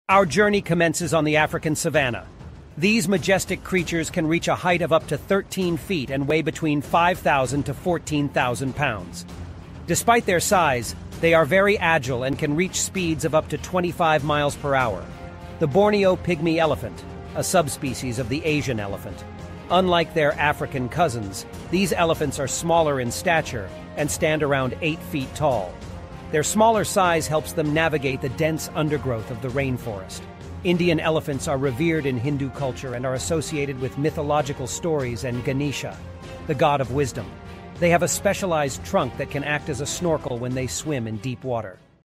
Elephant Kingdom
Join us on an epic journey as we explore the mesmerizing world of elephants! In this documentary-style video, we dive deep into the diverse species of elephants, from the colossal African Savannah Elephant to the elusive Forest Elephant of Africa's rainforests and the Desert Elephants thriving in Namibia's arid deserts. Discover fascinating facts, stunning visuals, and the incredible adaptability of these gentle giants.

Learn about their intelligence, unique traits, and the cultural significance they hold in various parts of the world. From the snowy Himalayan foothills to the tropical paradise of Sri Lanka, this video celebrates the beauty and wonder of our planet's biodiversity. Don't miss this unforgettable adventure into the world of elephants!